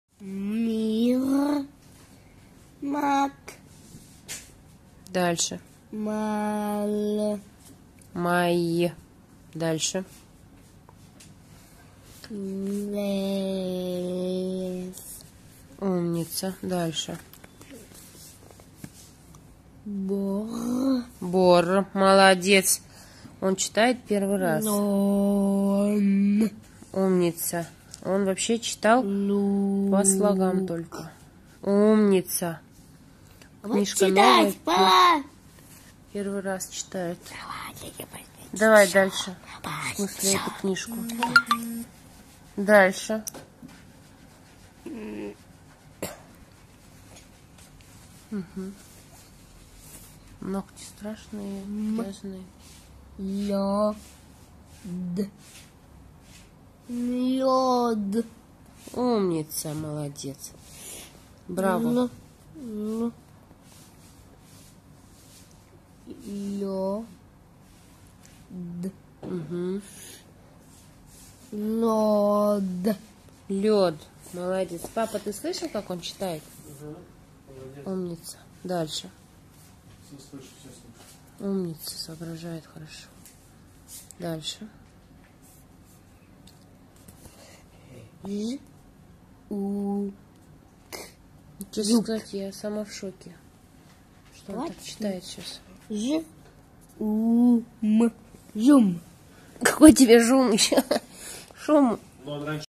Ахан читает в первый раз. Центр "Беэр Давид", Иерусалим
Во время лечения в Израиле, в Центре "Беэр Давид", Ахан научился читать целыми словами!